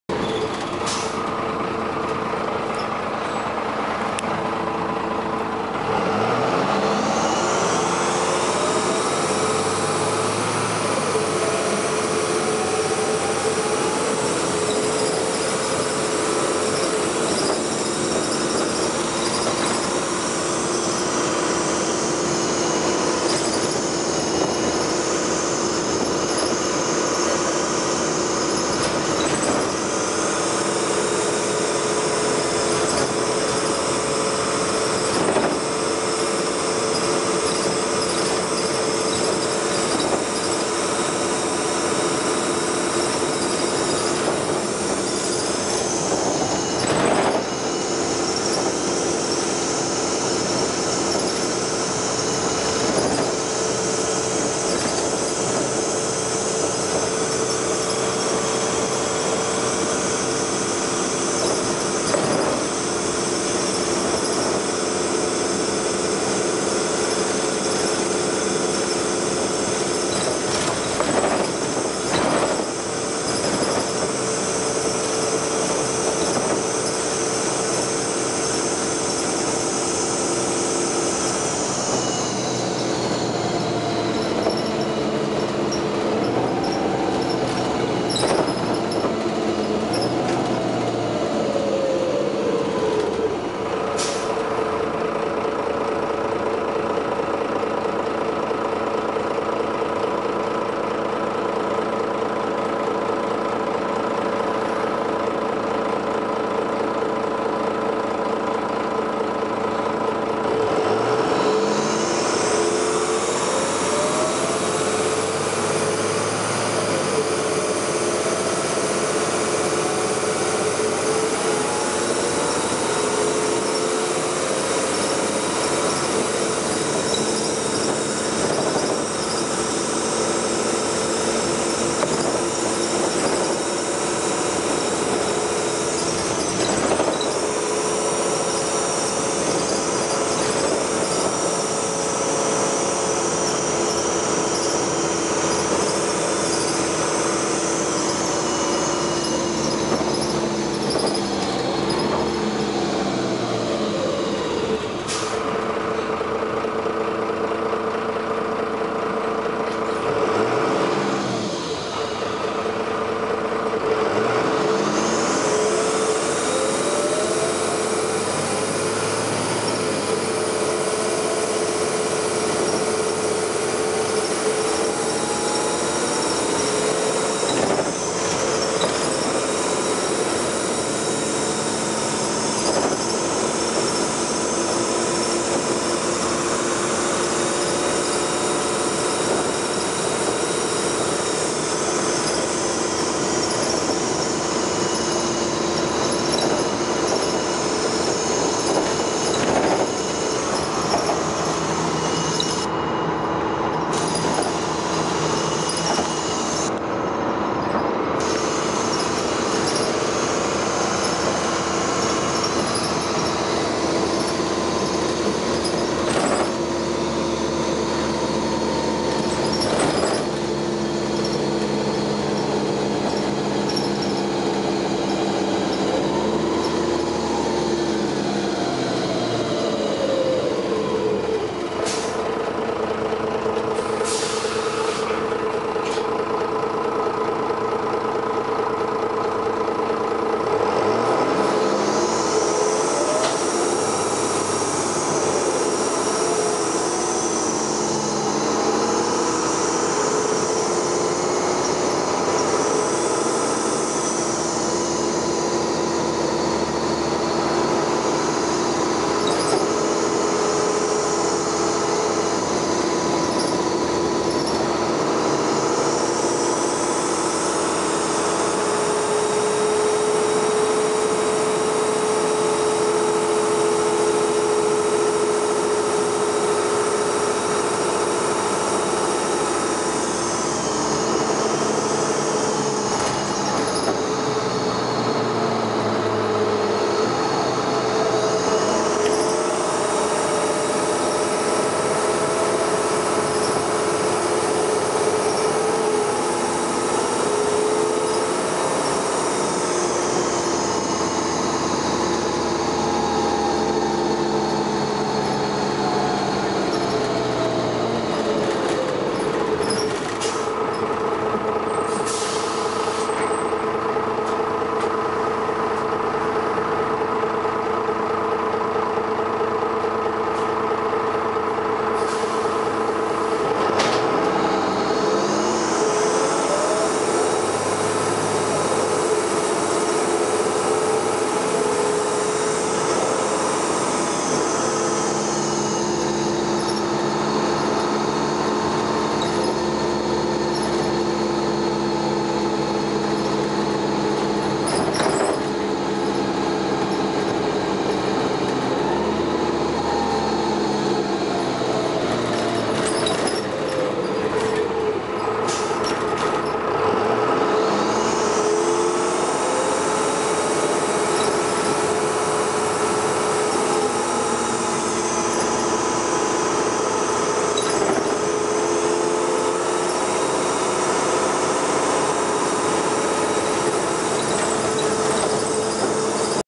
First Manchester Optare Excel 60151 R882ENF
First Manchester Optare Excel, which has strange noise, and also with a very nice Allison! Recorded 18th September 2012.